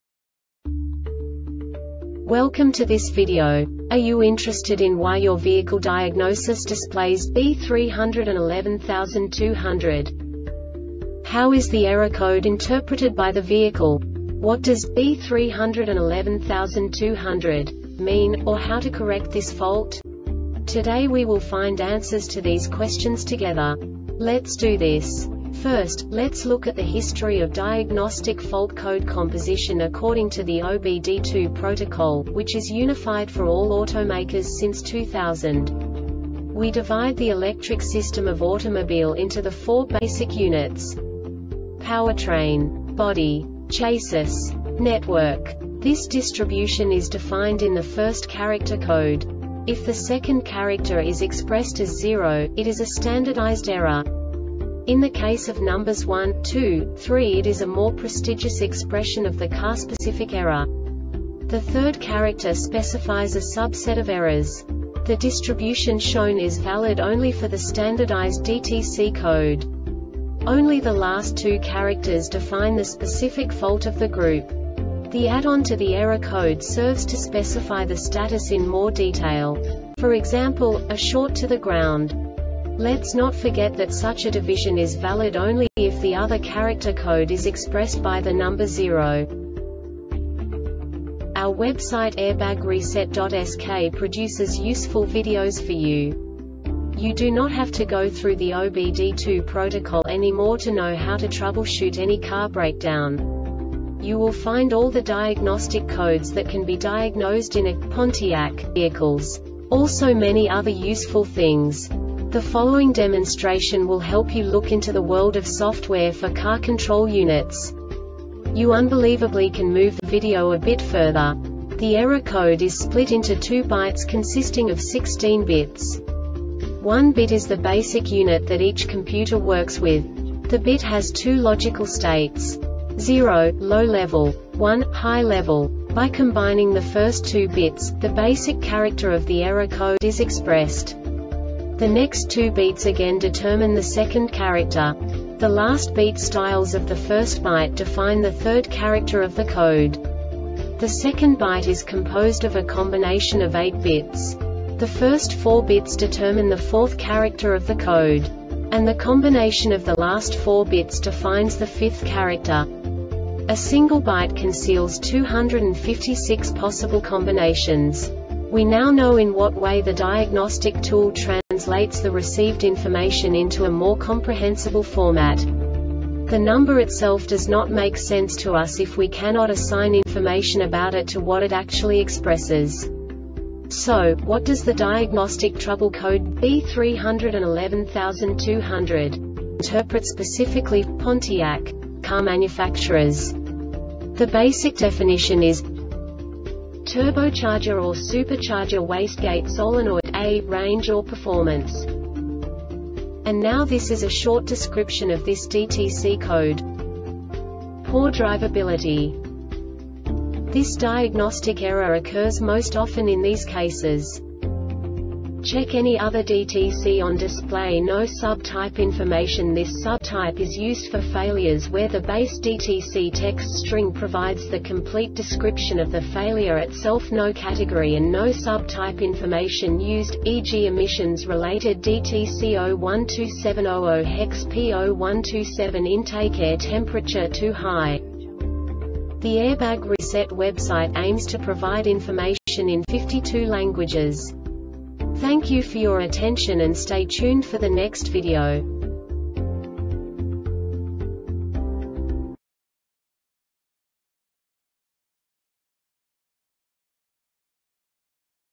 DTC Pontiac B3112-00 Short Explanation
Contents:
0:21 Basic DTC analysis according to OBD2 protocol standard.
1:48 Insight into programming
2:58 A diagnostically understandable description of the Diagnostic Trouble Code
3:05 Basic error definition

Make:
Pontiac
Code:
B3112 00
Definition:
Turbocharger or Supercharger Wastegate Solenoid “A” Range or Performance
Description:
Poor driveability.
Cause:
 Check any other DTC on display
Failure Type: